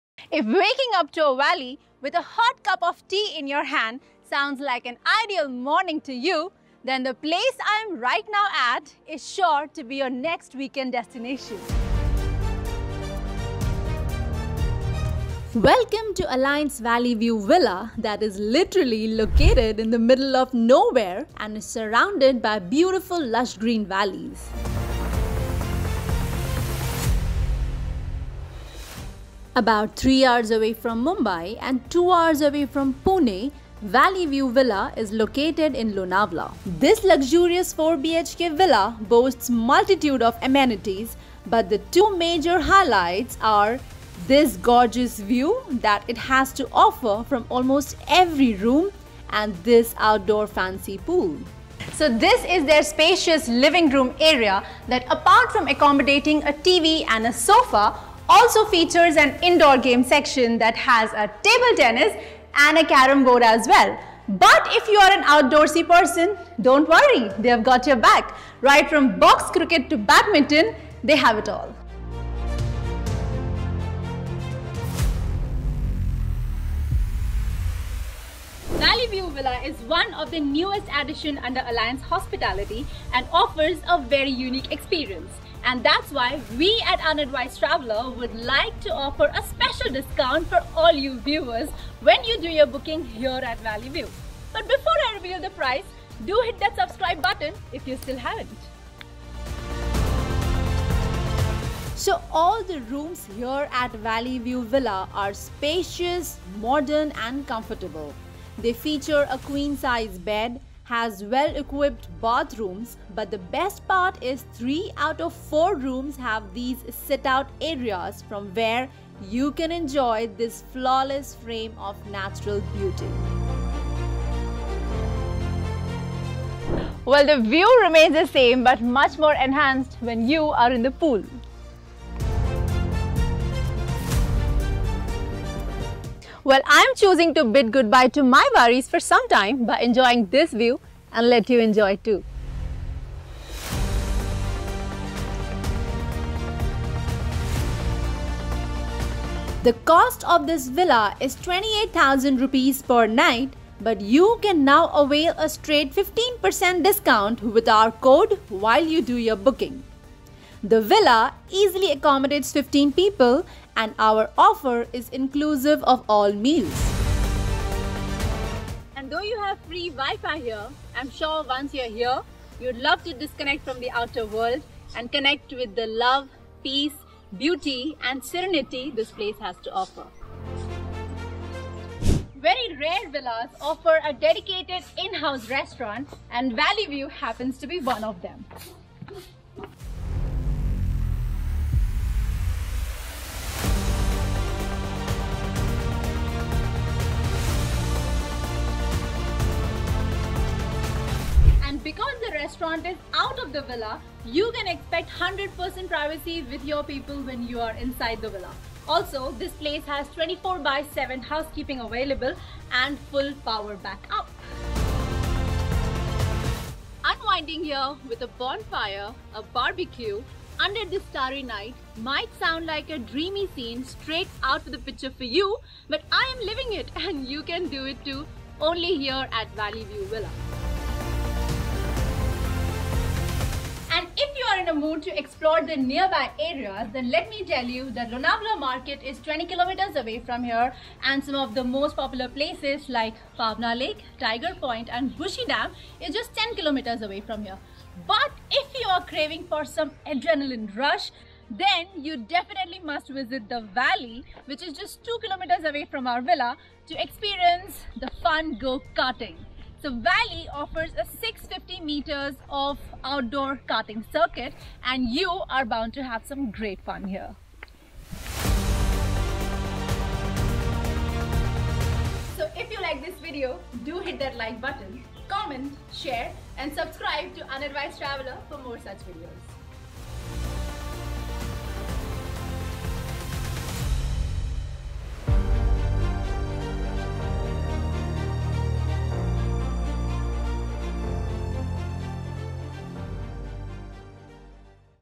A villa with a private pool overlooking the valley | Lonavala | Unadvised Traveller
Craving for a staycation with a private pool that overlooks a valley?

'Alliance Valley View Villa' is the luxurious 4BHK villa in Lonavala that is your perfect getaway from the heatwaves in the city!

ATTENTION!!! The current rate in the season time for this villa is INR 45,000 for 10 people including all meals. Kindly get in touch with us accordingly.